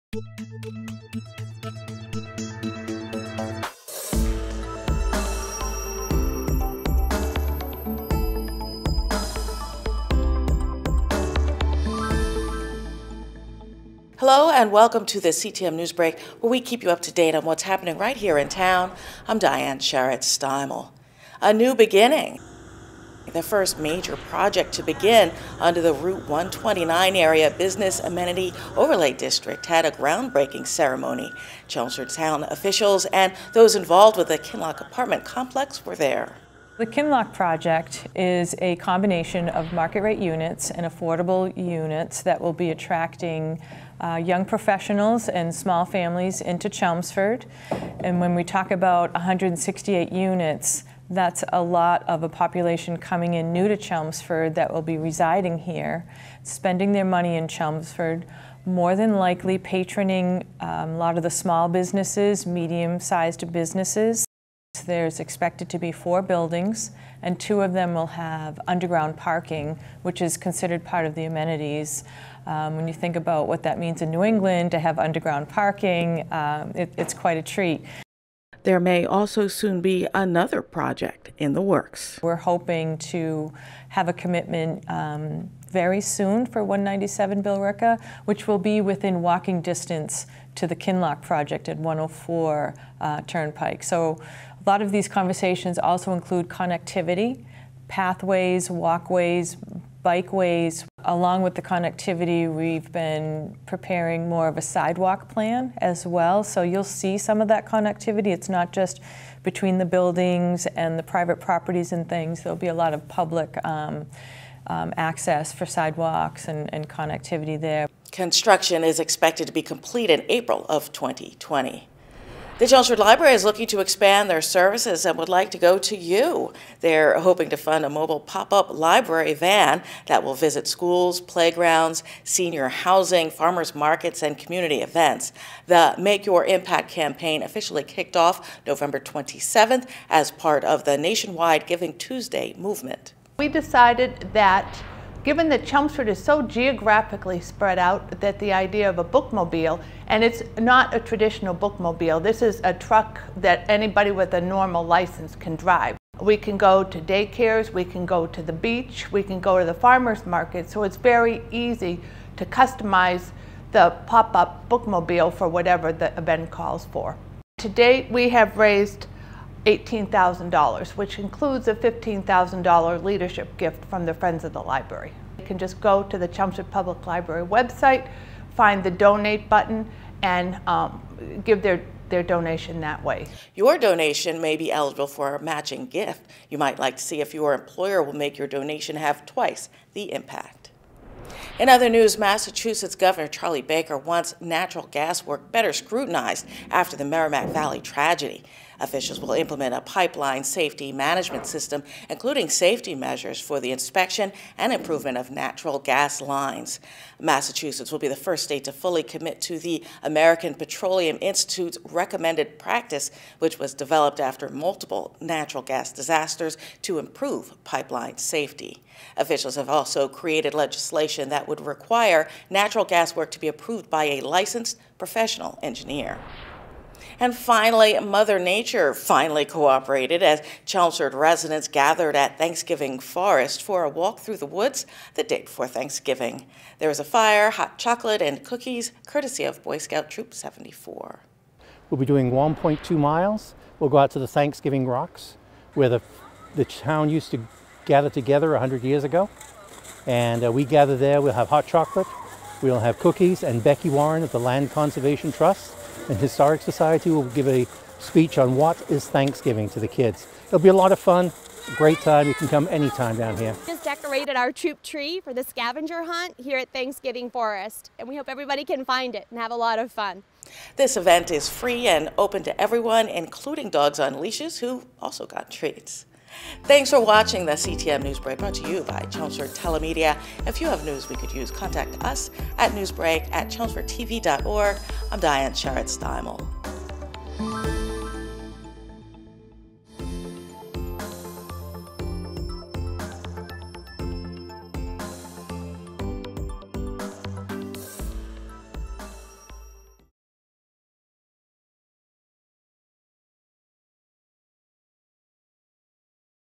CTM NewsBreak: December 5, 2018
In this episode:
The first major project to begin under the Route 129 area Business Amenity Overlay District has a ground breaking ceremony - Lisa Marrone, the Chelmsford Library is hoping to fund a mobile Pop-up Library van - Maggie Marshall, Massachusetts Gov. Charlie Baker wants natural gas work better scrutinized after the Merrimack Valley tragedy and area residents gather at Thanksgiving Forest for a walk through the woods the day before Thanksgiving - Phil Stanway.